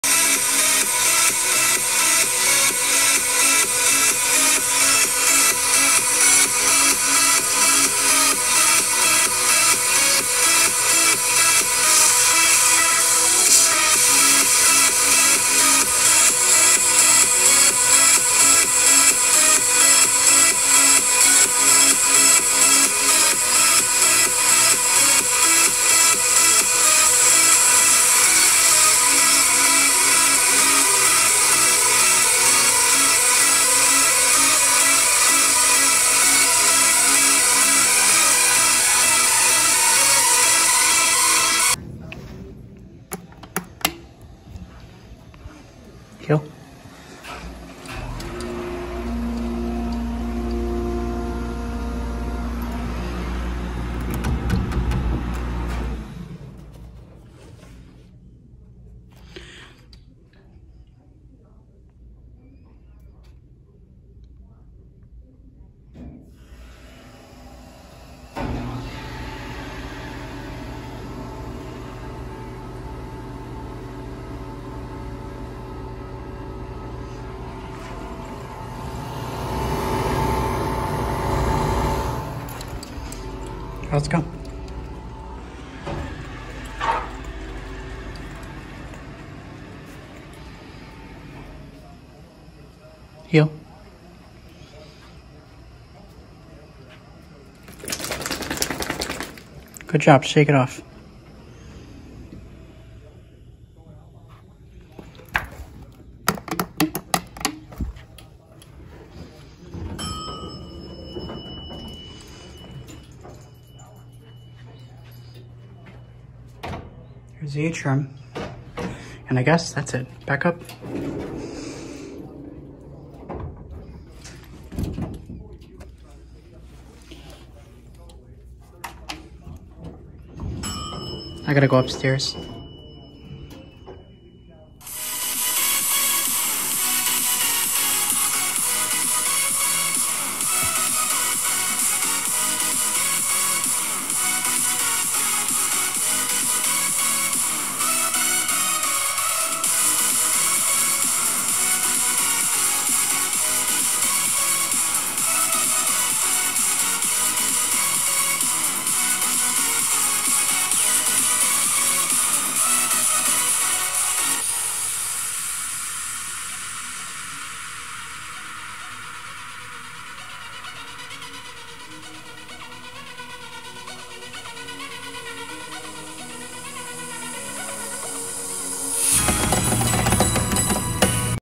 The Motor Pitches sound slightly different on 2 Montgomery Hydraulic Elevators-Machine Room!
Like how the pitches sound slightly different